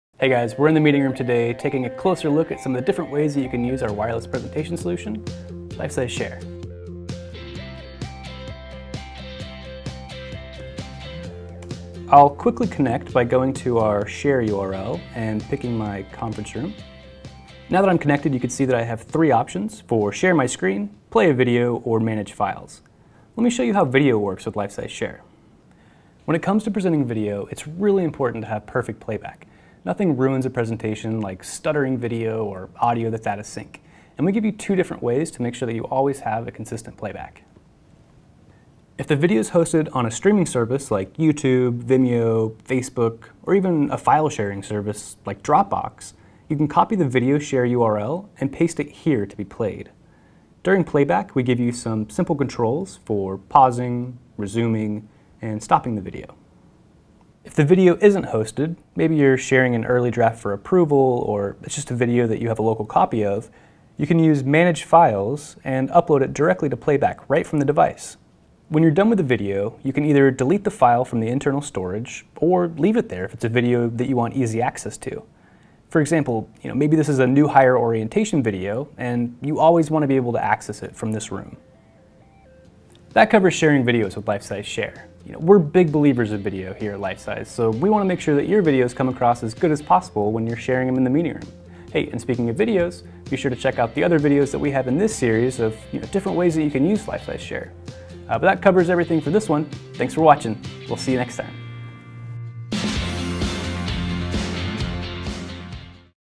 Lifesize Share | Reproducción de un Video
En este video, le mostramos cómo presentar videos de forma inalámbrica en la sala de reuniones. Simplemente conéctese a Lifesize Share, seleccione su sala de reuniones y podrá reproducir videos desde una URL compartida o desde la utilidad de administración de archivos.

Nada arruina una presentación como el tartamudeo de video o problemas de sincronización de audio, por lo que diseñamos nuestra solución para brindarle la mejor reproducción de video inalámbrico posible para su sala de reuniones.